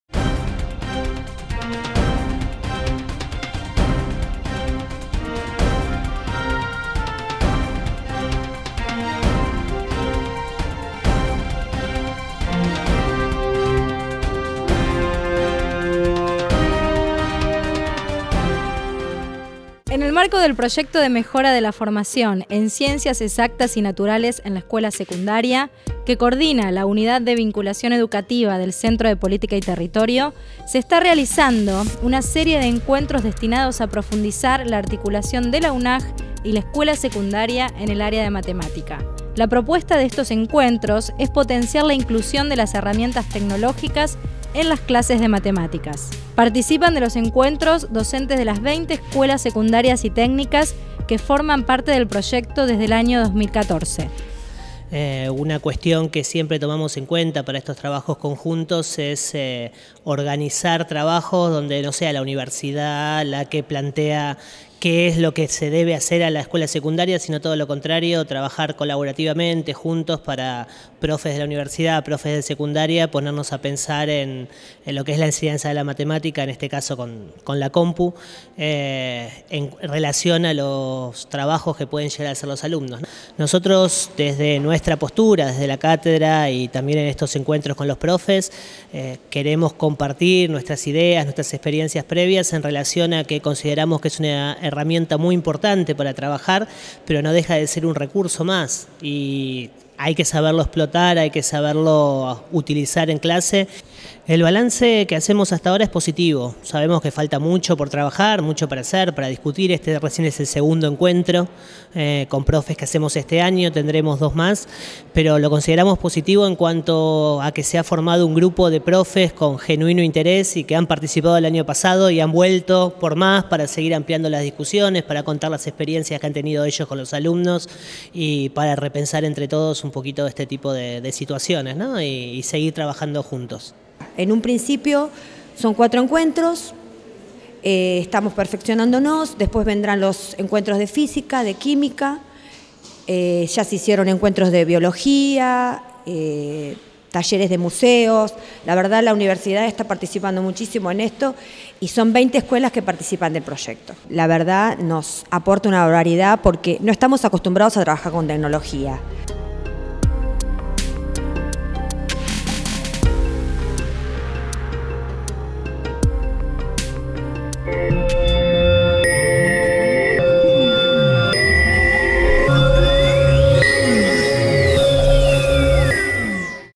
Capacitación Matemática y TICs
La propuesta de estos encuentros en la UNAJ, es potenciar la inclusión de las herramientas tecnológicas en las clases de matemáticas en escuelas secundarias.